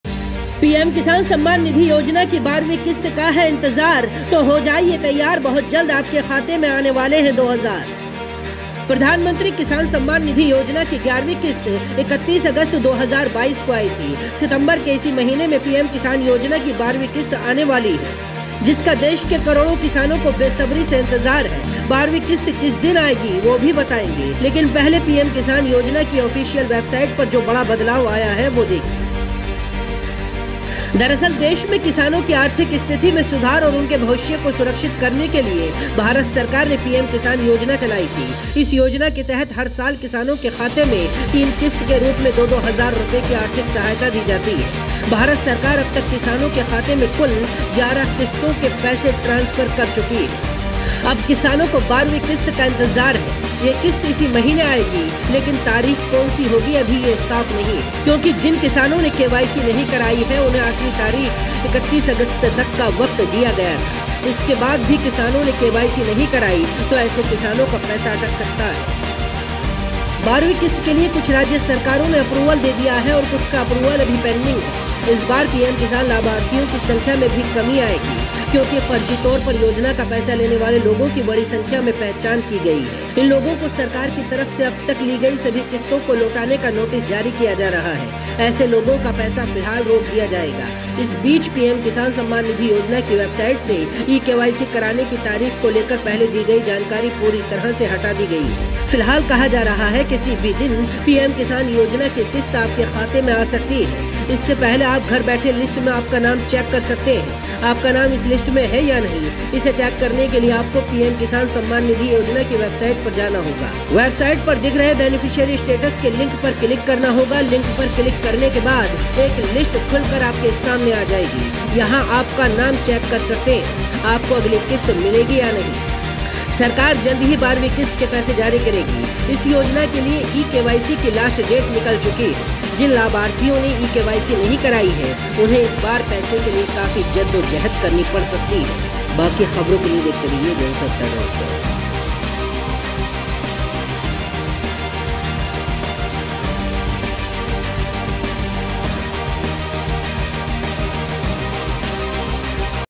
PM Kisan Yojana: पीएम किसान सम्मान निधि की 12वीं किस्त का है इंतजार, जानिए किस दिन आएंगे दो हजार ?
PM Kisan Samman Nidhi Yojana: प्रधानमंत्री किसान सम्मान निधि योजना की 12वीं किस्त का बेसब्री से इंतजार किया जा रहा है, बहुत जल्द ये किस्त आपके खाते में आने वाली है.